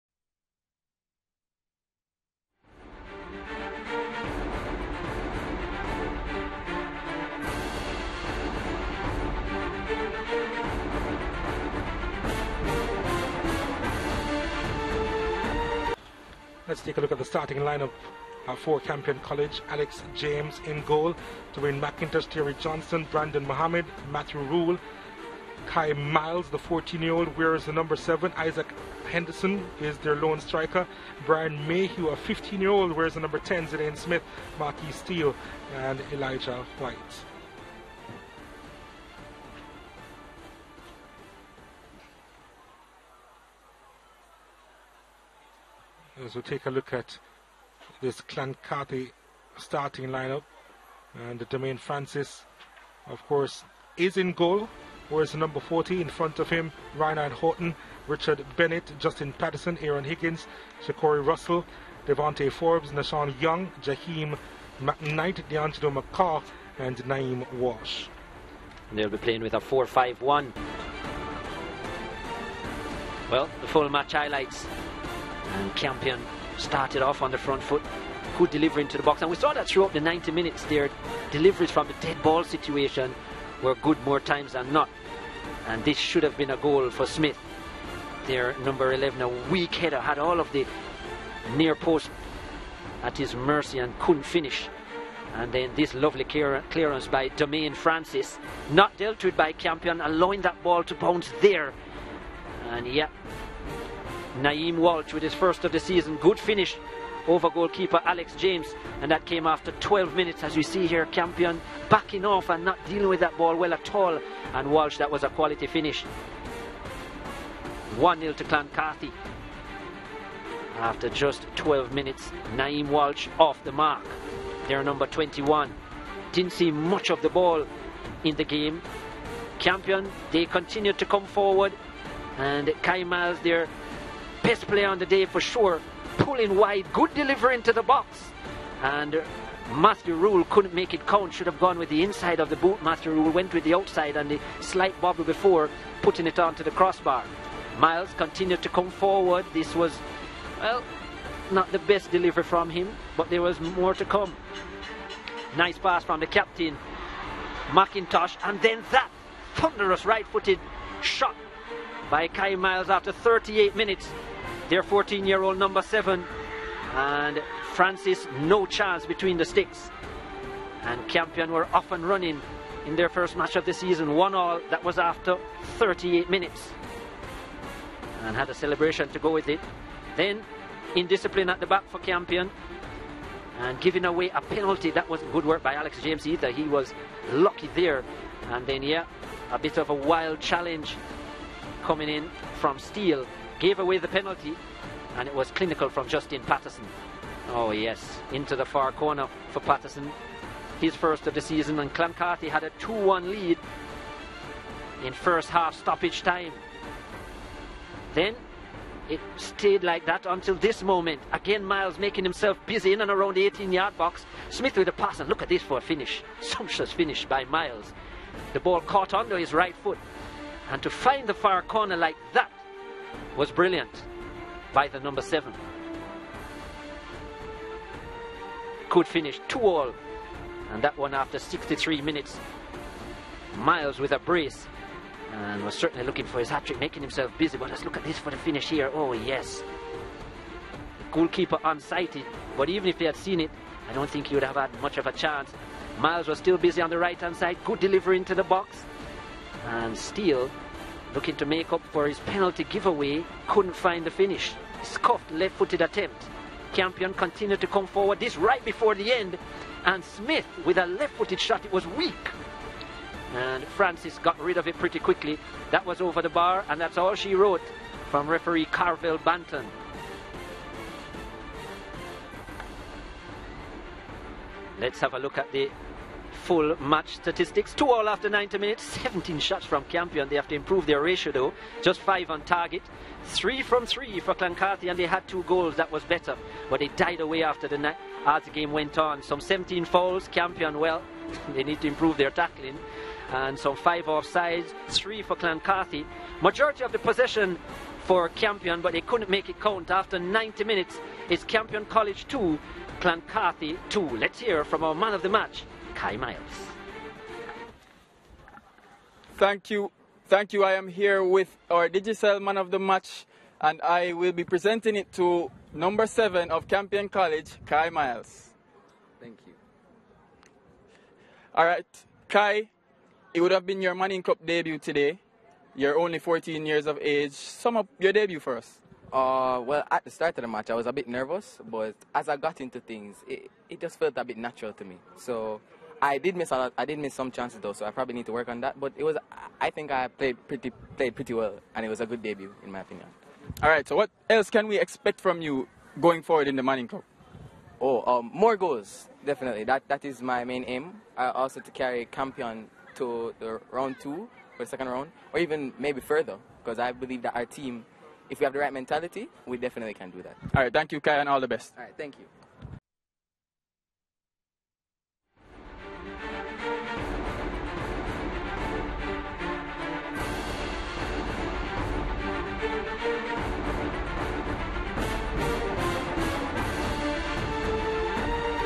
Kai Myles impresses as Campion draw 2-2 with Clan Carthy in Manning Cup Round 1! | SportsMax TV
SportsMax TV presents full highlights of Campion College vs Clan Carthy High in Manning Cup Round 1 in the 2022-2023 SchoolBoy Football season.

Watch ALL Schoolboy Football matches live for free on the SportsMax app.
 Watch SBF live games on TV on SportsMax and CEEN.